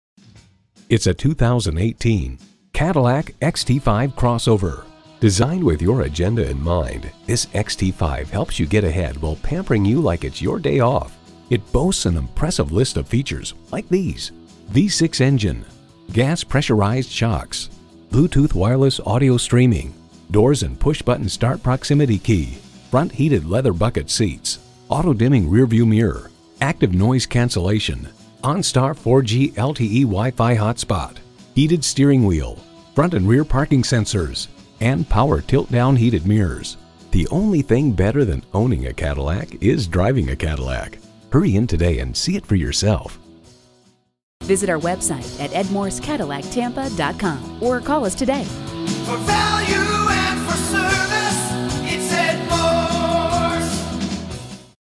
New 2018 Cadillac XT5 Tampa FL St. Petersburg, FL JZ243791 - SOLD
Year: 2018
Make: Cadillac
Model: XT5
Trim: Luxury
Engine: 3.6l 6 Cylinder Fuel Injected
Transmission: FWD 8-Speed Shiftable Automatic
Color: White
Interior: Tan
Stock #: JZ243791
VIN: 1GYKNCRSXJZ243791
Recent Arrival! 2018 Cadillac XT5 Luxury 26/19 Highway/City MPG

Address:
101 East Fletcher Avenue